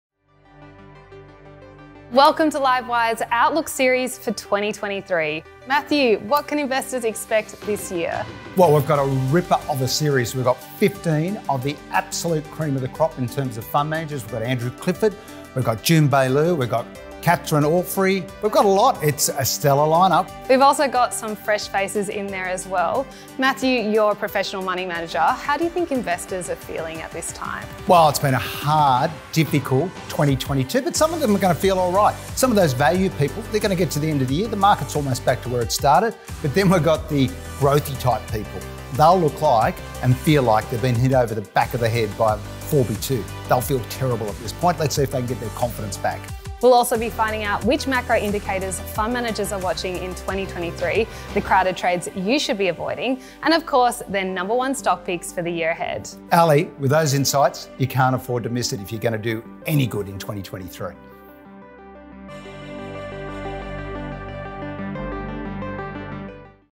Check out Livewire's Outlook Series 2023
Matthew Kidman (Centennial Asset Management) and Ally Selby (Livewire Markets) were thrilled to interview 15 of Australia's most respected fund managers on their:

- Worst investment in 2022
- What macro indicators they'll be watching in 2023
- Best performing stock predictions for 2023
- Who among their peers they would invest their own cash with
- Plus more...

Head to the page below to see the first of some of the great content from the series. And if you are not a subscriber already, you can sign up for free to access investment ideas and strategies from Australia’s leading investors.